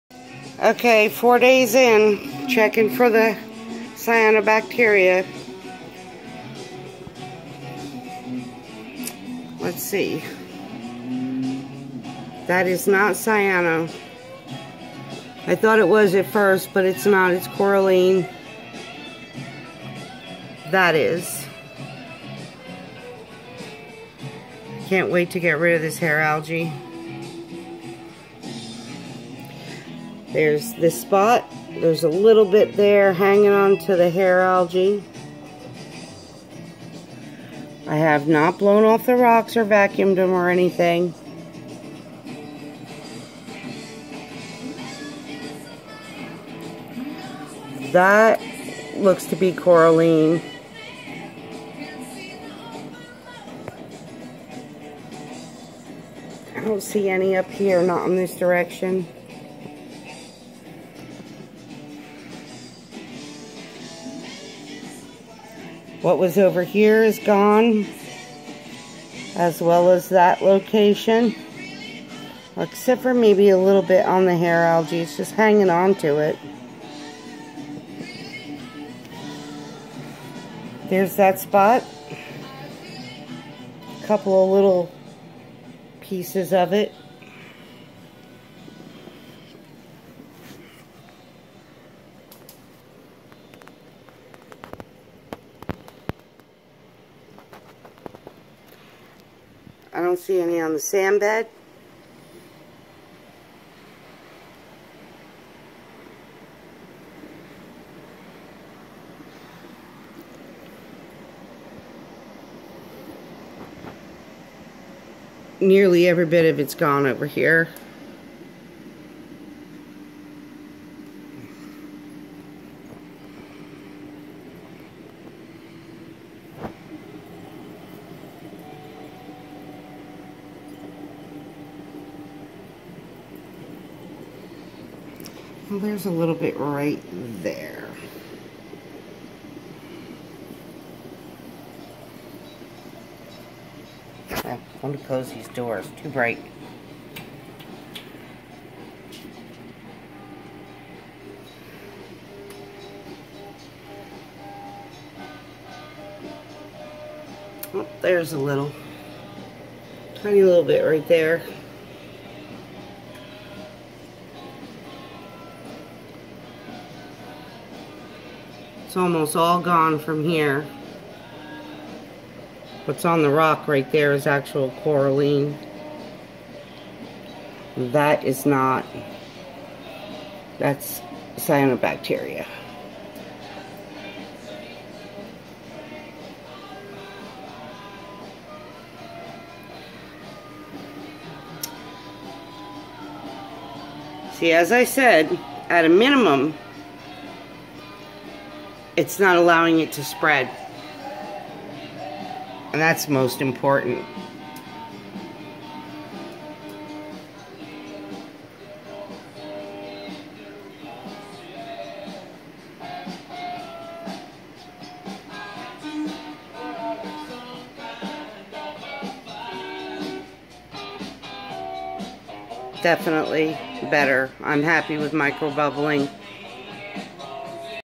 Micro-bubbles for cyanobacteria #3
4 days in... I call it a success, or so it is with my 180. Still working on ha removal, on the 5th dose of Vibrant. I'm told I should start seeing obvious results soon... hope so!